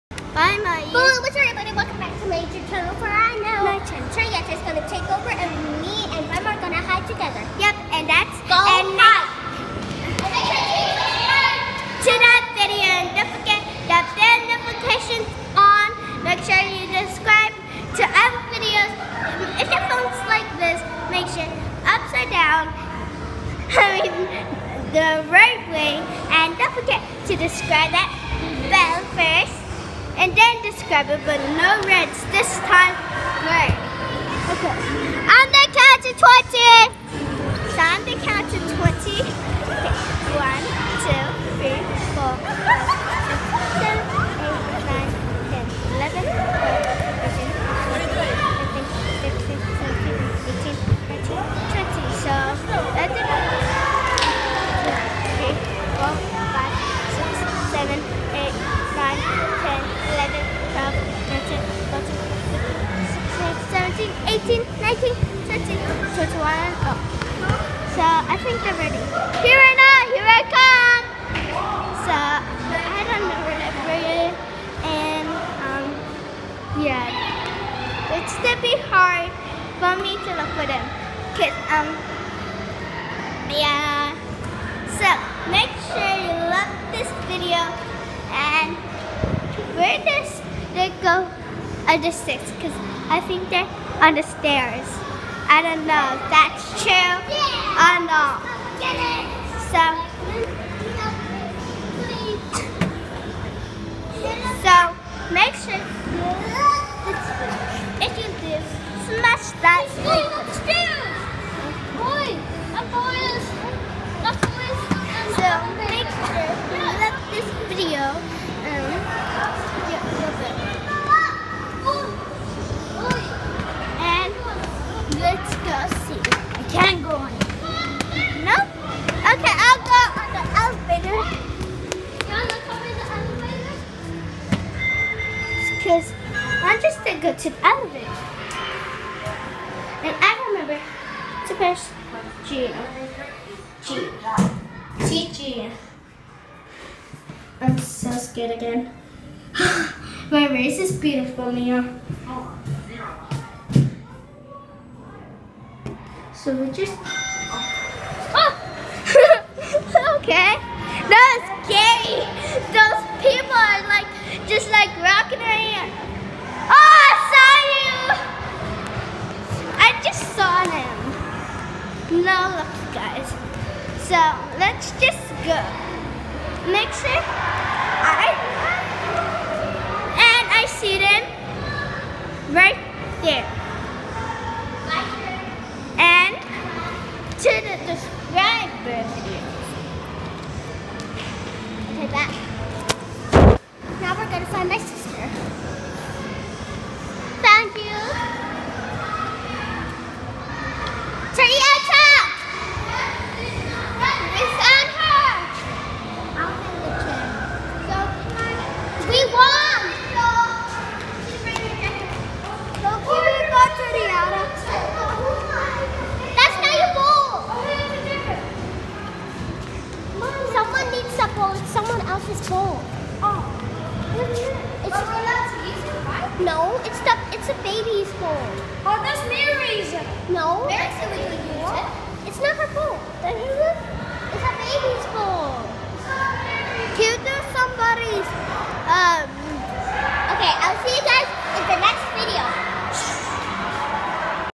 Playing hide and seek with my sister and my friend
Go and play the SOY NINJA NETWORK! KICK BUMP!!!